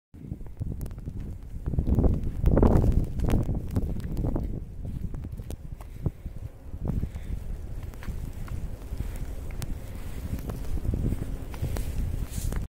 What it feels like in raining snow
10th April around 7pm MST in the University. Windy, snowy.